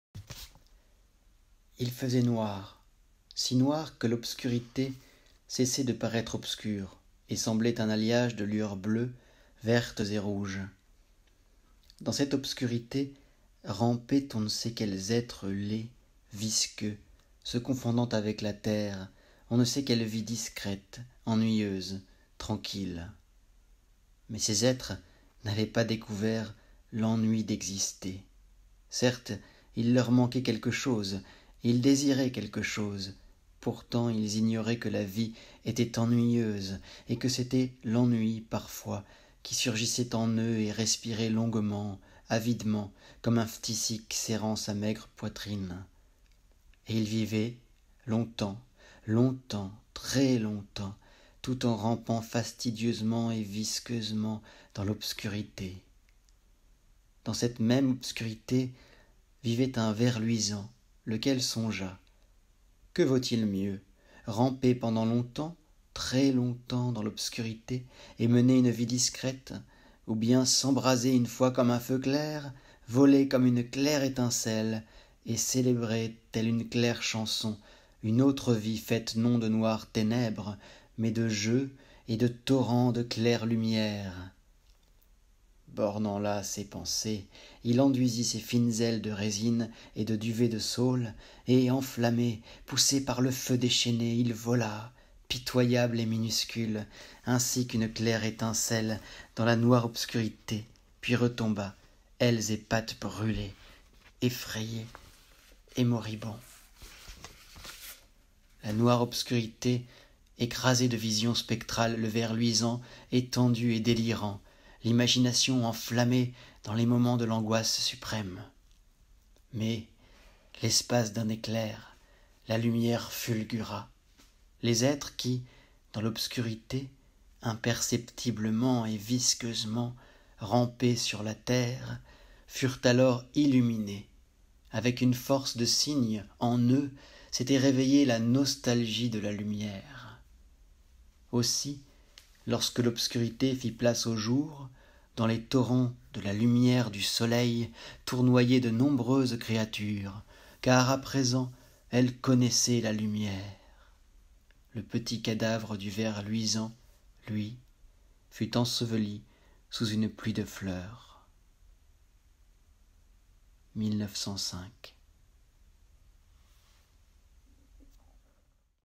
KHLEBNIKOV - Vers luisant (Nouvelles du Je et du Monde)
Lecture d'un poème en prose de Velimir Khlebnikov, sans titre, issu du recueil Nouvelles du Je et du Monde  (édition inconnue ? pas celle récente des oeuvre complètes)

Dixième lecture du confinement.